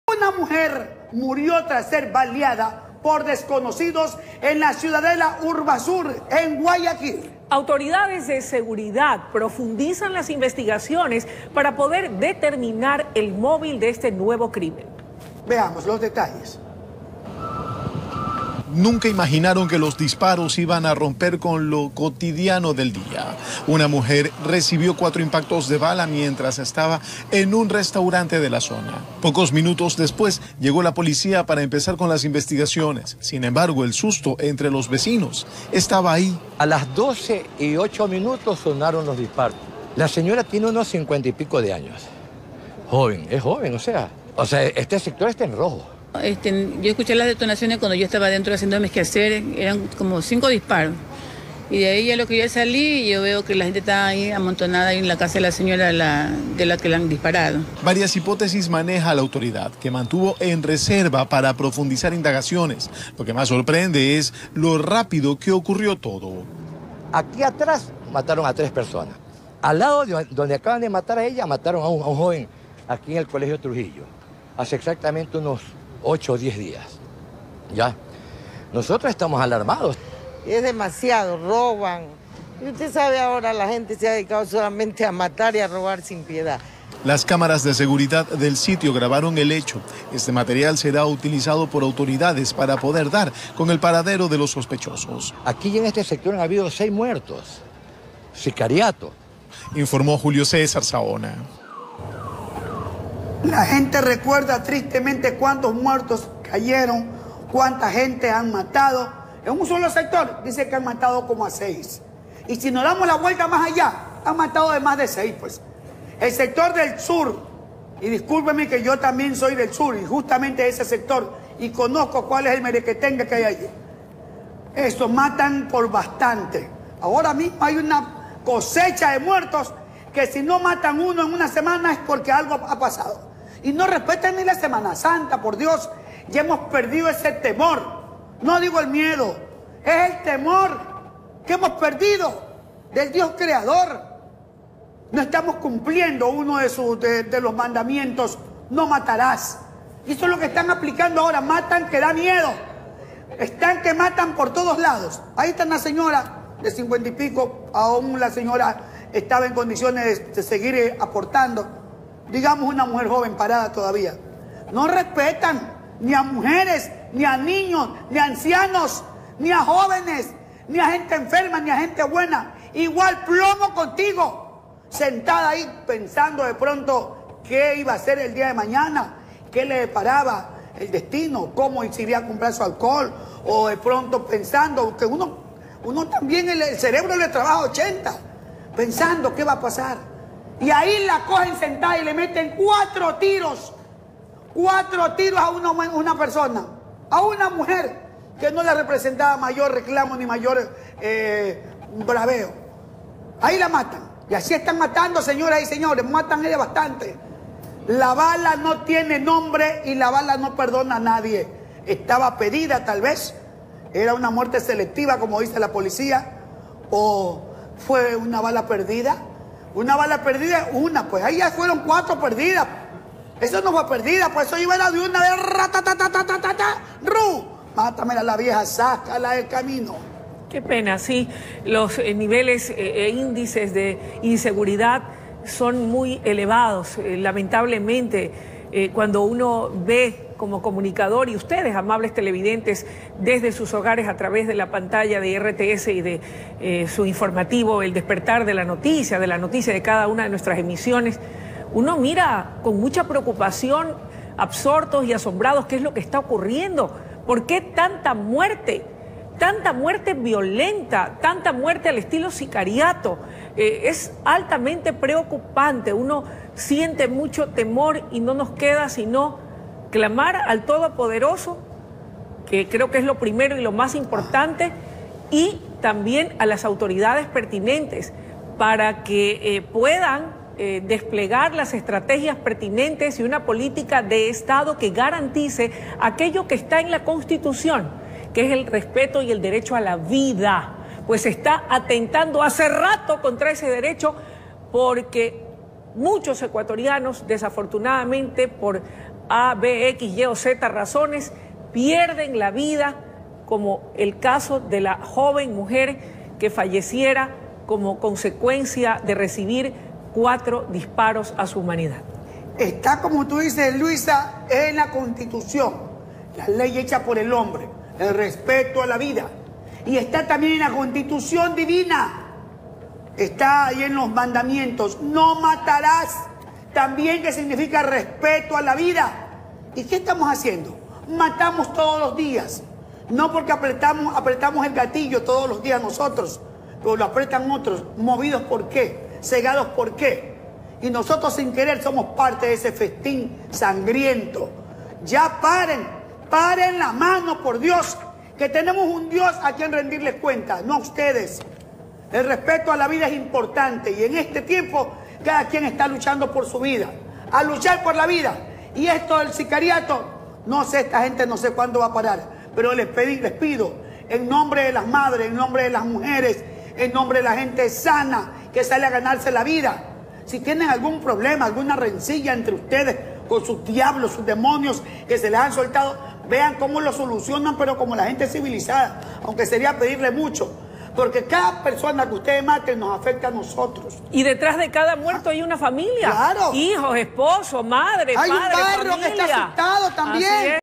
Una mujer fue asesinada tras ser baleada por sujetos desconocidos
Este nuevo hecho de sangre se registró en un sector de la ciudadela Urbasur, Guayaquil

Las autoridades policiales informaron que profundizan las investigaciones, para poder determinar el móvil de este nuevo crimen.